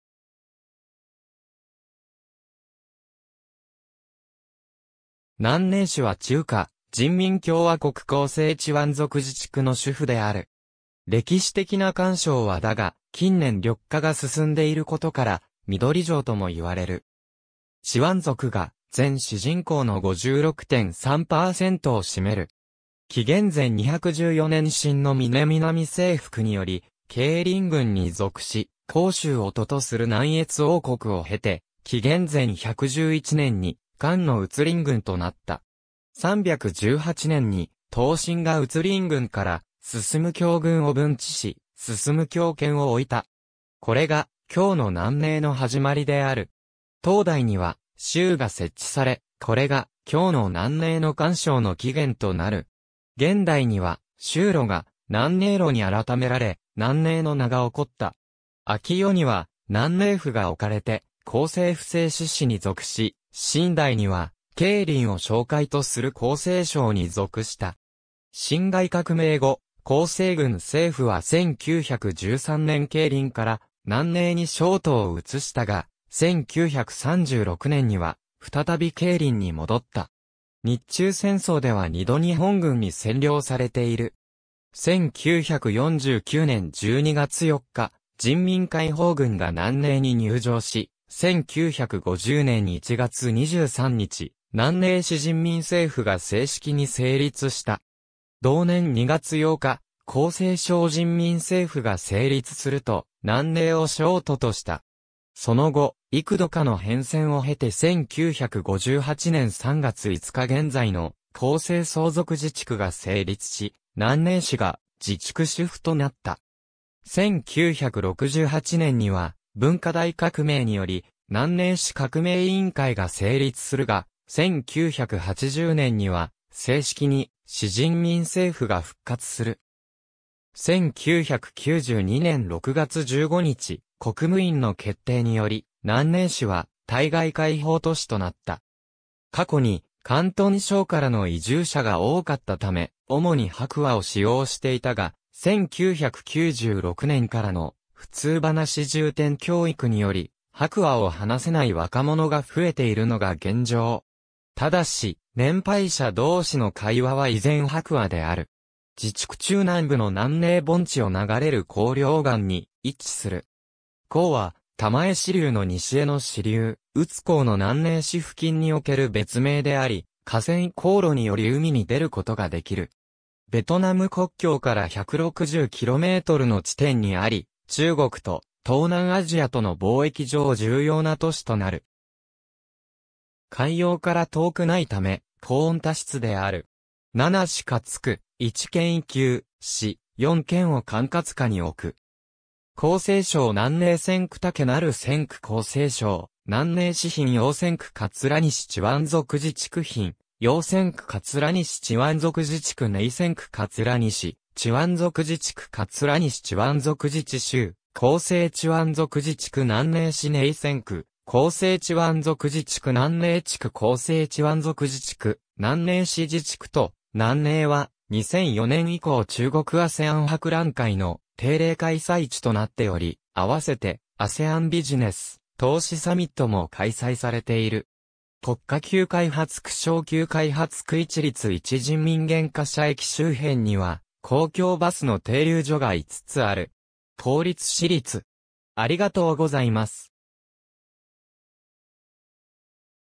南寧市
南寧市（なんねいし、簡体字中国語: 南宁市、チワン語: Namzningz）は中華人民共和国広西チワン族自治区の首府である。
歴史的な簡称は邕（よう）だが、近年緑化が進んでいることから緑城とも言われる。
チワン族が全市人口の 56.3 パーセントを占める。
紀元前214年秦の嶺南征服により桂林郡に属し、広州を都とする南越王国を経て、紀元前111年に漢の鬱林郡となった。
318年に東晋が鬱林郡から晋興郡を分置し、晋興県を置いた。
これが今日の南寧の始まりである。
唐代には邕州が設置され、これが今日の南寧の簡称の起源となる。
元代には邕州路が南寧路に改められ、南寧の名が起った。
明代には南寧府が置かれて、広西布政使司に属し、清代には桂林を省会とする広西省に属した。
辛亥革命後、広西軍政府は1913年桂林から南寧に省都を移したが、1936年には再び桂林に戻った。
日中戦争では二度日本軍に占領されている。
1949年12月4日人民解放軍が南寧に入城し、1950年1月23日南寧市人民政府が正式に成立した。
同年2月8日広西省人民政府が成立すると南寧を省都とした。
その後、幾度かの変遷を経て1958年3月5日現在の広西壮族自治区が成立し、南寧市が自治区首府となった。
1968年には文化大革命により南寧市革命委員会が成立するが、1980年には正式に市人民政府が復活する。
1992年6月15日、国務院の決定により南寧市は対外開放都市となった。
過去に広東省（特に中山市）からの移住者が多かったため、主に白話（広東語）を使用していたが、1996年からの普通話重点教育により白話を話せない若者が増えているのが現状。
ただし、年配者同士の会話は依然白話である。
自治区中南部の南寧盆地を流れる邕江両岸に位置する。
邕江は珠江支流の西江の支流・鬱江の南寧市付近における別名であり、河川航路により海に出ることができる。
ベトナム国境から 160 キロメートルの地点にあり、中国と東南アジアとの貿易上重要な都市となる。
海洋から遠くないため、高温多湿である。
7市轄区・1県級市・4県を管轄下に置く。
広西省南寧専区 武鳴専区 広西省南寧市 賓陽専区 桂西チワン族自治区賓陽専区 桂西チワン族自治区邕寧専区 桂西チワン族自治区 桂西チワン族自治州 広西チワン族自治区南寧市(第1次) 邕寧専区 広西チワン族自治区南寧地区 広西チワン族自治区南寧市(第2次) 自治区都・南寧は、2004年以降中国ASEAN博覧会（CAEXPO）の定例開催地となっており、併せてASEANビジネス・投資サミットも開催されている。
国家級開発区 省級開発区 一律1人民元（BRT、エアコンバスは2人民元）火車駅周辺には、公共バスの停留所が5つある。
公立 私立 （日本語）